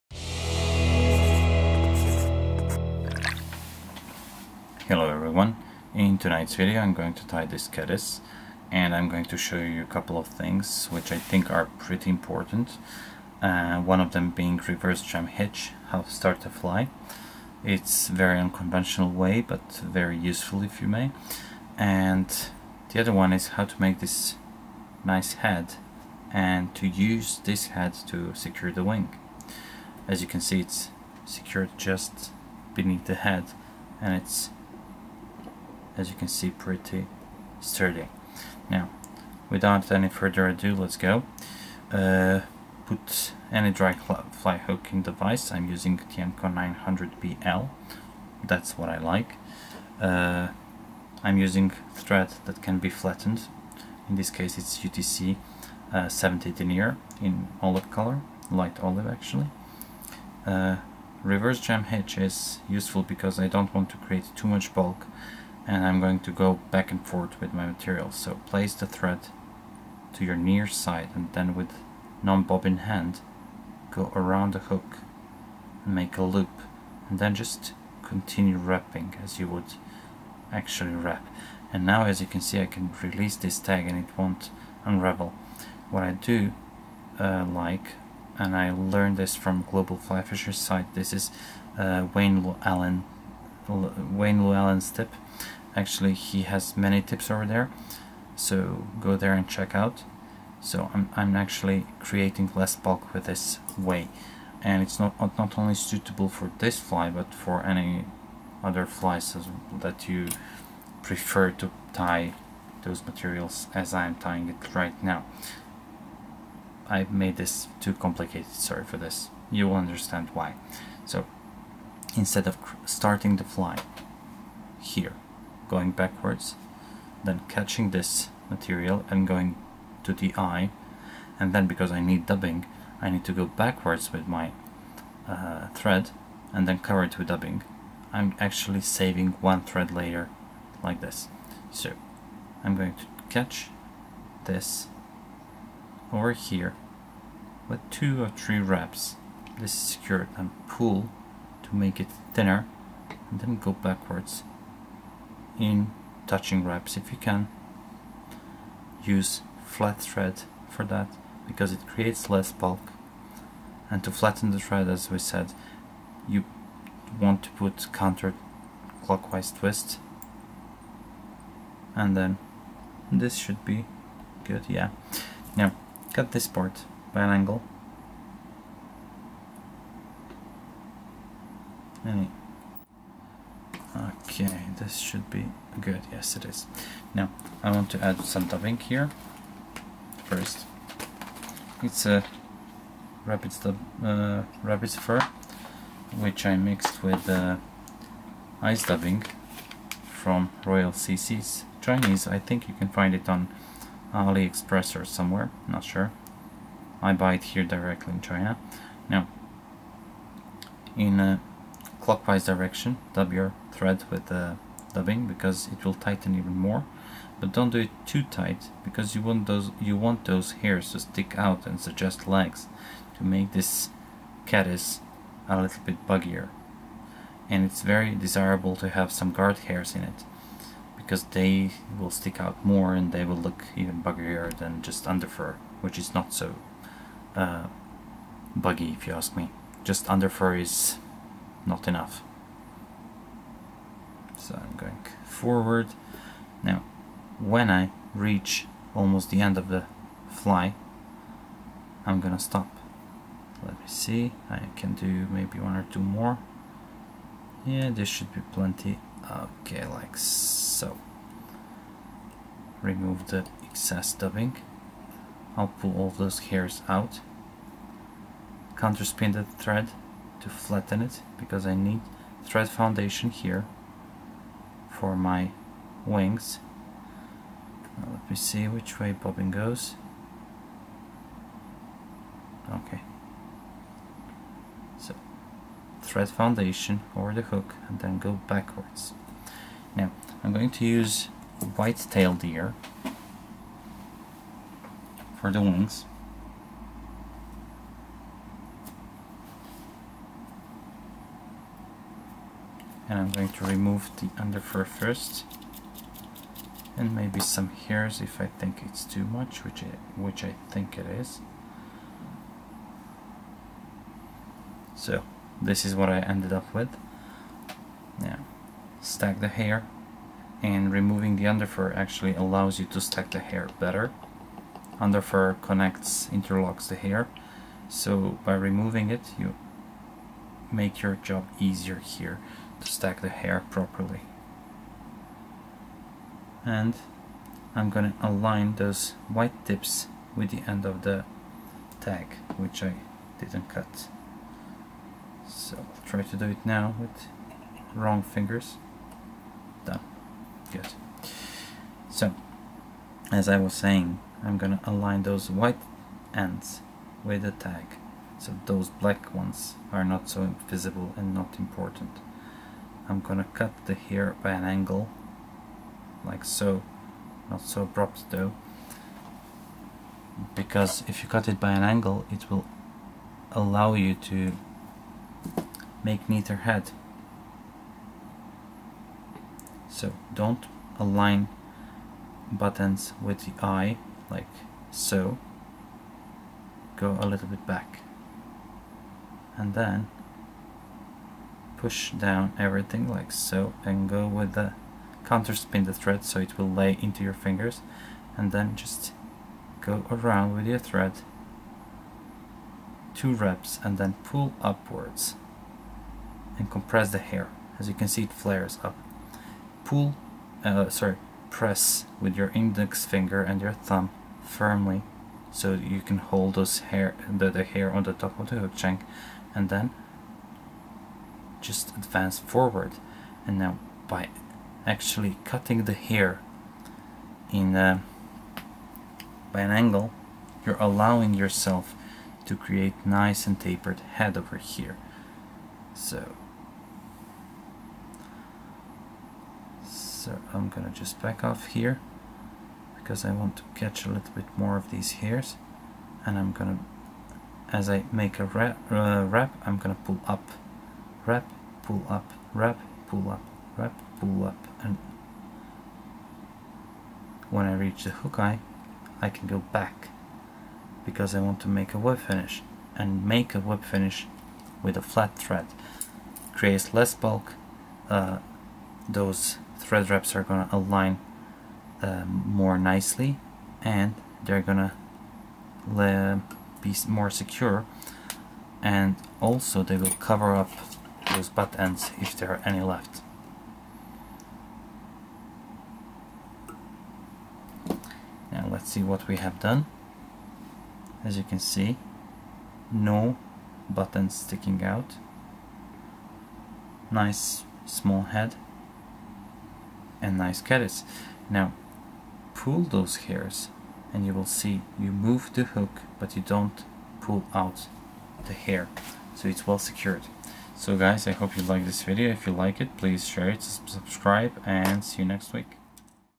Elk Caddis (Covered butt ends and reversed jam hitch)
Elk caddis is basic pattern for many fly fisherman. Its simple, trout love it.
This one was created to sit low in the water film. Position that tells fish the food is not going to fly away any second and its an easy meal.
I am also using reversed jam hitch to start the fly and the main point is covering but ends neatly.

Both techniques can be found on globalflyfisher website under Wayne Luallen's tips...

Cheers

Vladimir